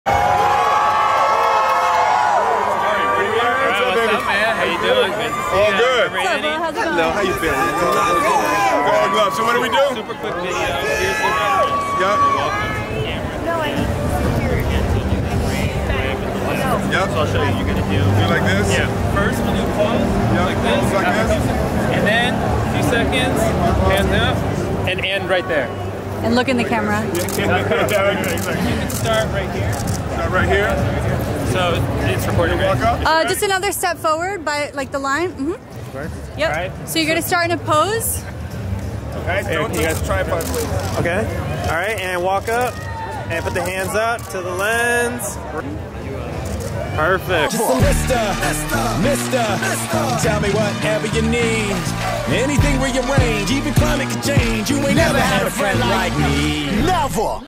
We had 45 seconds to shoot with Will Smith...
Back in 2019, me and Cache Bunny were asked to shoot a video for Will Smith during the Aladdin red carpet premiere. We knew we'd have limited time with him, so we came up with a concept and filming plan that would allow us to shoot as quickly and efficiently as possible. Our video was posted on all of Will's social accounts to help promote the release of Disney's Aladdin.